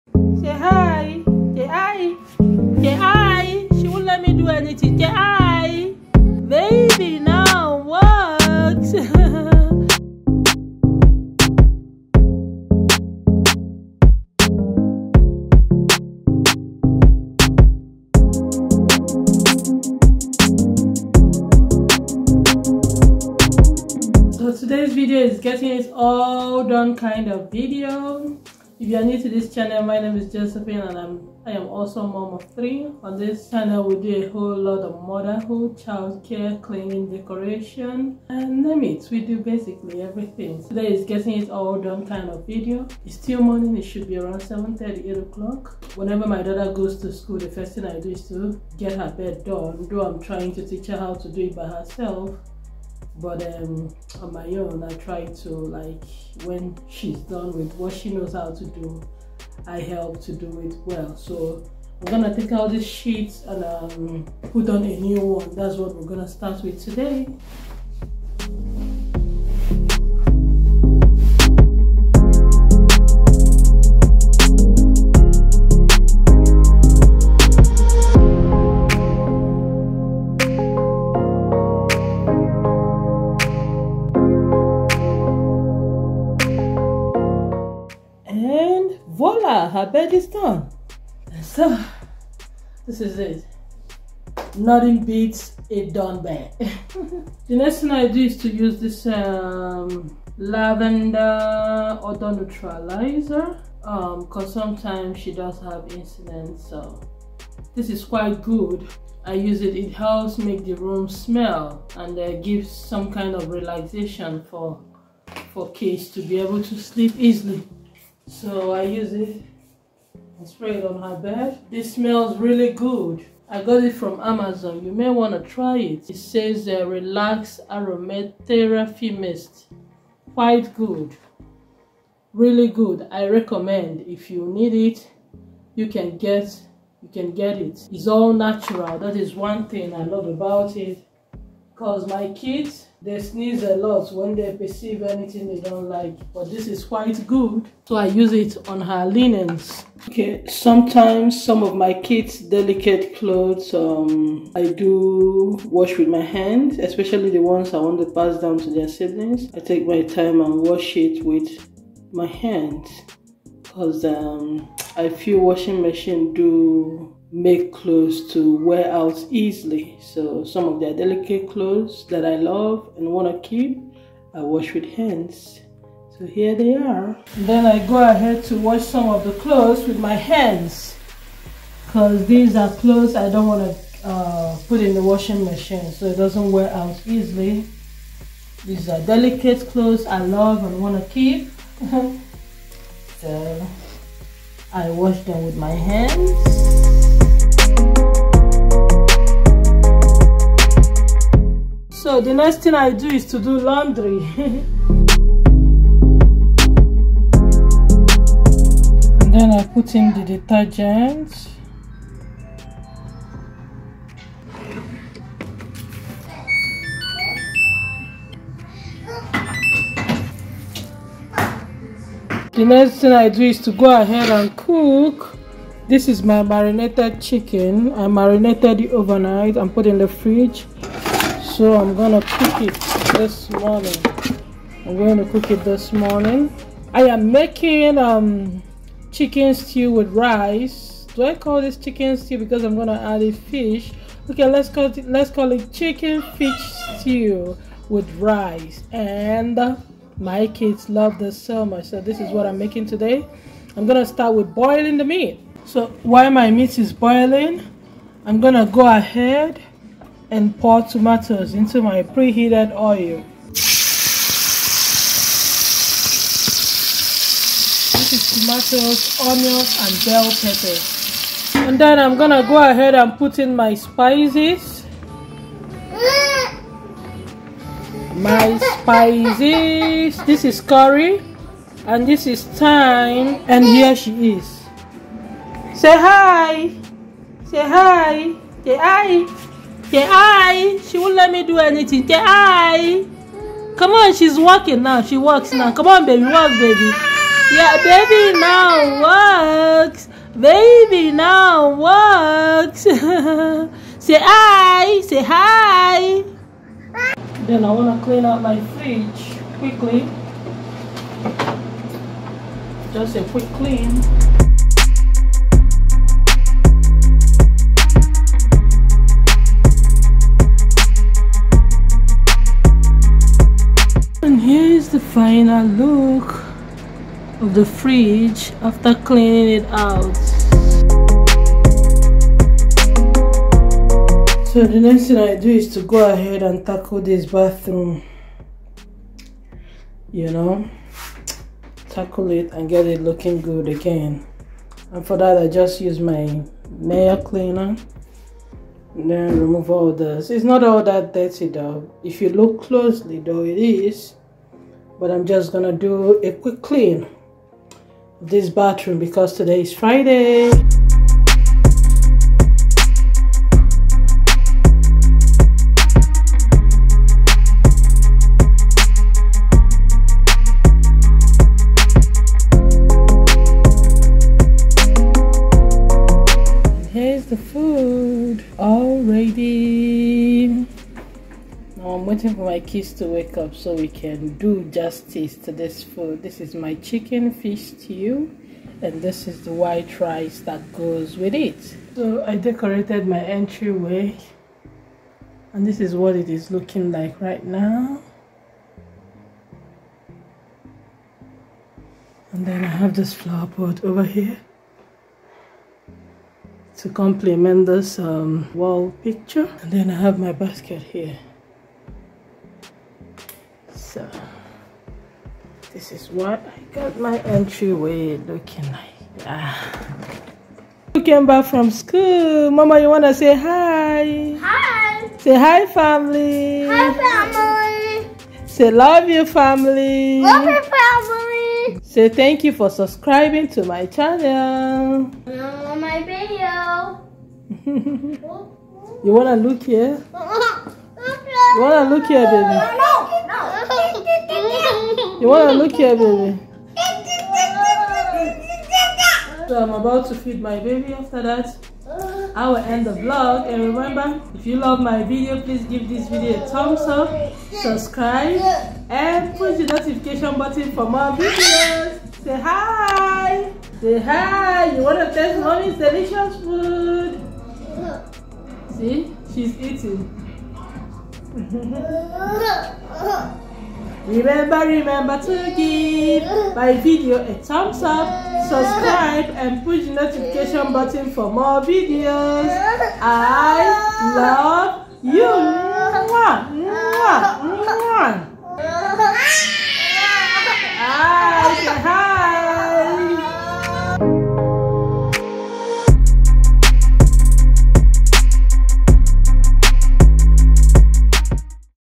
Get it all done, cleaning, decoration and cooking
Get your to do list and let us tackle the chores together. This video will motivate you to get up and clean up that space you dread cleaning